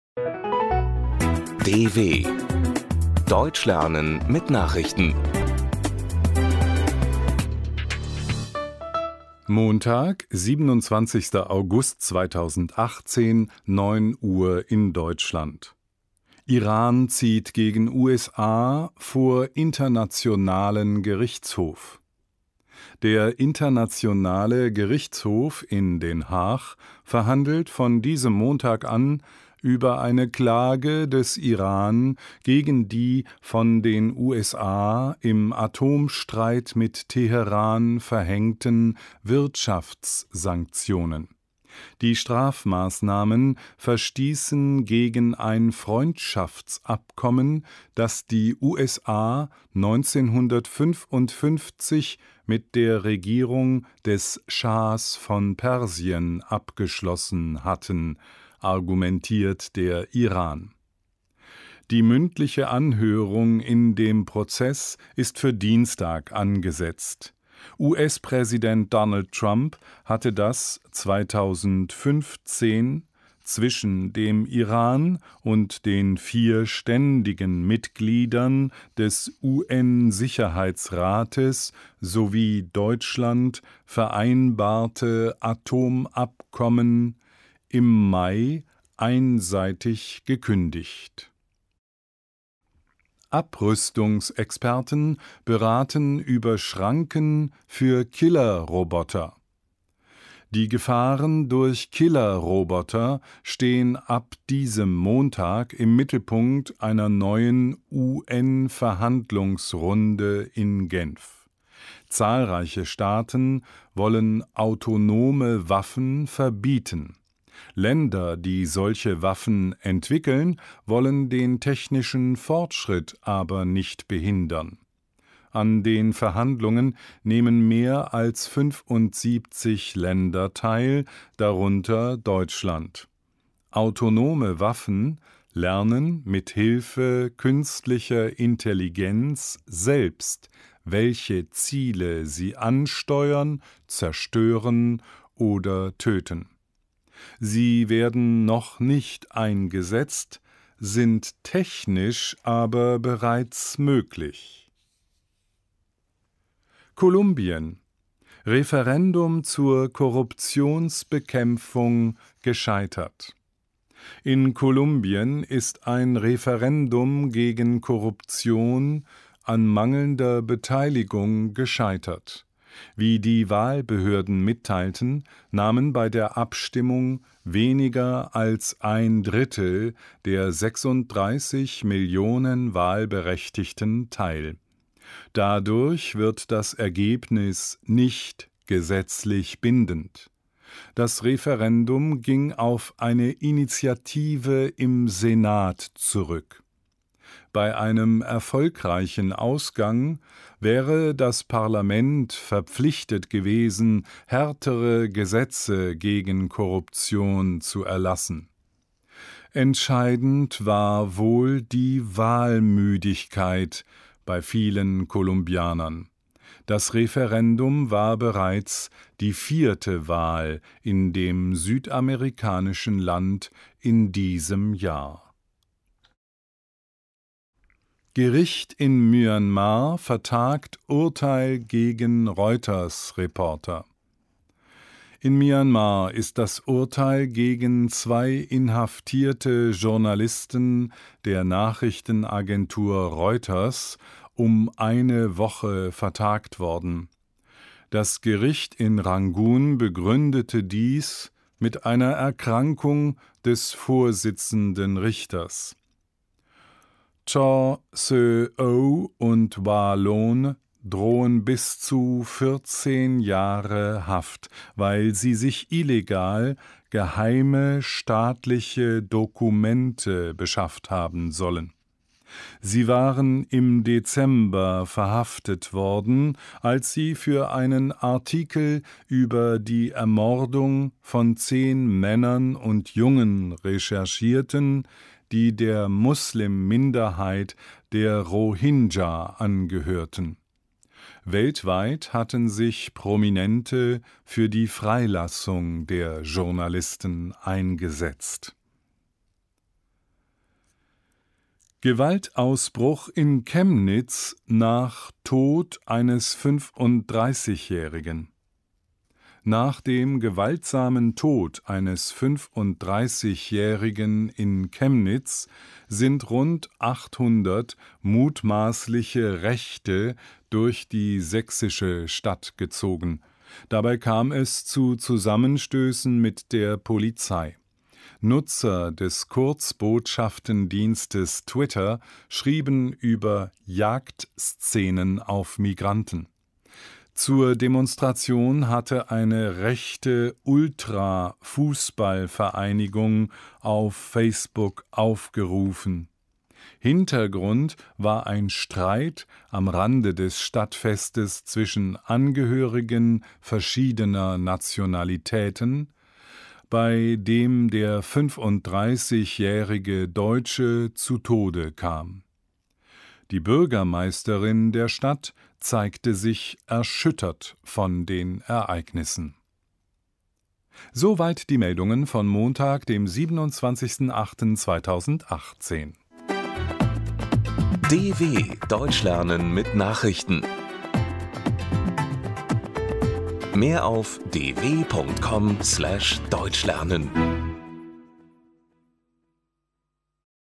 Deutsch lernen mit Nachrichten, 27 08 2018 – langsam gesprochen
Iran zieht gegen USA vor Internationalen Gerichtshof

Der Internationale Gerichtshof in Den Haag verhandelt von diesem Montag an über eine Klage des Iran gegen die von den USA im Atomstreit mit Teheran verhängten Wirtschaftssanktionen. Die Strafmaßnahmen verstießen gegen ein Freundschaftsabkommen, das die USA 1955 mit der Regierung des Schahs von Persien abgeschlossen hatten, argumentiert der Iran. Die mündliche Anhörung in dem Prozess ist für Dienstag angesetzt. US-Präsident Donald Trump hatte das 2015 zwischen dem Iran und den vier ständigen Mitgliedern des UN-Sicherheitsrates sowie Deutschland vereinbarte Atomabkommen im Mai einseitig gekündigt.

Abrüstungsexperten beraten über Schranken für Killerroboter

Die Gefahren durch Killerroboter stehen ab diesem Montag im Mittelpunkt einer neuen UN-Verhandlungsrunde in Genf. Zahlreiche Staaten wollen autonome Waffen verbieten. Länder, die solche Waffen entwickeln, wollen den technischen Fortschritt aber nicht behindern. An den Verhandlungen nehmen mehr als 75 Länder teil, darunter Deutschland. Autonome Waffen lernen mit Hilfe künstlicher Intelligenz selbst, welche Ziele sie ansteuern, zerstören oder töten. Sie werden noch nicht eingesetzt, sind technisch aber bereits möglich.

Kolumbien: Referendum zur Korruptionsbekämpfung gescheitert

In Kolumbien ist ein Referendum gegen Korruption an mangelnder Beteiligung gescheitert. Wie die Wahlbehörden mitteilten, nahmen bei der Abstimmung weniger als ein Drittel der 36 Millionen Wahlberechtigten teil. Dadurch wird das Ergebnis nicht gesetzlich bindend. Das Referendum ging auf eine Initiative im Senat zurück. Bei einem erfolgreichen Ausgang wäre das Parlament verpflichtet gewesen, härtere Gesetze gegen Korruption zu erlassen. Entscheidend war wohl die Wahlmüdigkeit bei vielen Kolumbianern - das Referendum war bereits die vierte Wahl in dem südamerikanischen Land in diesem Jahr.

Gericht in Myanmar vertagt Urteil gegen Reuters-Reporter

In Myanmar ist das Urteil gegen zwei inhaftierte Journalisten der Nachrichtenagentur Reuters um eine Woche vertagt worden. Das Gericht in Rangun begründete dies mit einer Erkrankung des vorsitzenden Richters. Kyaw Soe Oo und Wa Lone drohen bis zu 14 Jahre Haft, weil sie sich illegal geheime staatliche Dokumente beschafft haben sollen. Sie waren im Dezember verhaftet worden, als sie für einen Artikel über die Ermordung von zehn Männern und Jungen recherchierten, die der Muslim-Minderheit der Rohingya angehörten. Weltweit hatten sich Prominente für die Freilassung der Journalisten eingesetzt.

Gewaltausbruch in Chemnitz nach Tod eines 35-Jährigen

Nach dem gewaltsamen Tod eines 35-Jährigen in Chemnitz sind rund 800 mutmaßliche Rechte durch die sächsische Stadt gezogen. Dabei kam es zu Zusammenstößen mit der Polizei. Nutzer des Kurzbotschaftendienstes Twitter schrieben über Jagdszenen auf Migranten. Zur Demonstration hatte eine rechte Ultra-Fußballvereinigung auf Facebook aufgerufen. Hintergrund war ein Streit am Rande des Stadtfestes zwischen Angehörigen verschiedener Nationalitäten, bei dem der 35-Jährige Deutsche zu Tode kam. Die Bürgermeisterin der Stadt zeigte sich erschüttert von den Ereignissen.